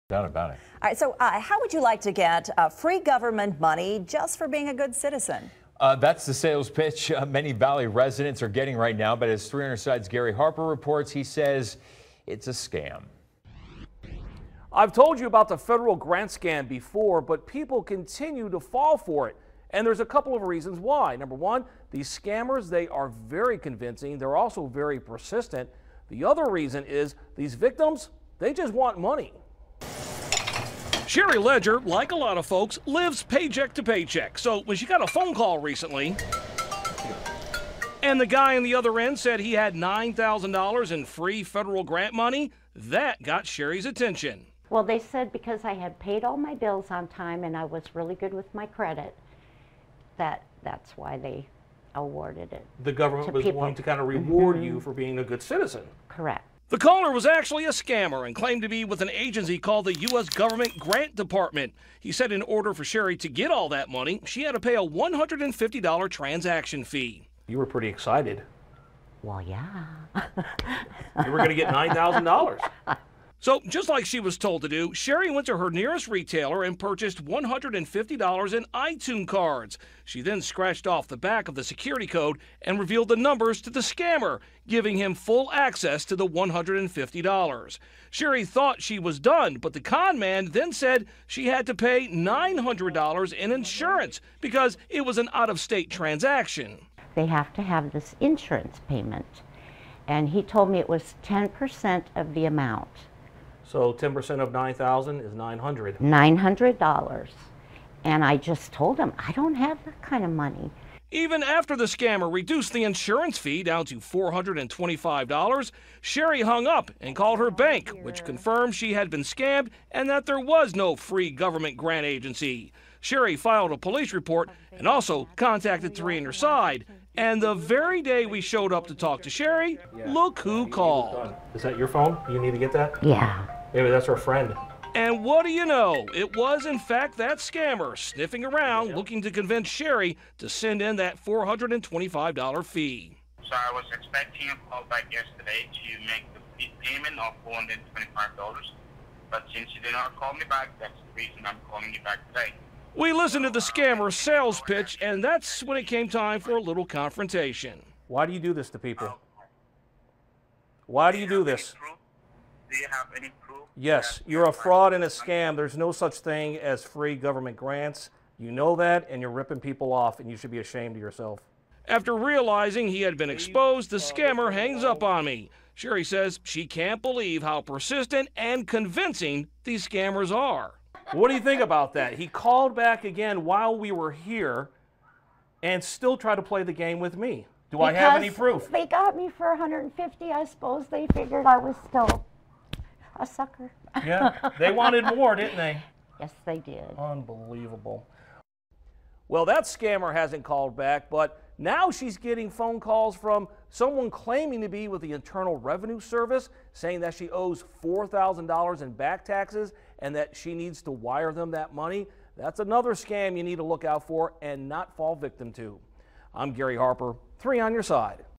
Don't fall for the 'Federal Grant Scam'
A Valley woman said a caller promised her free government grant money but it was all a scam. (Monday, May 15, 2017)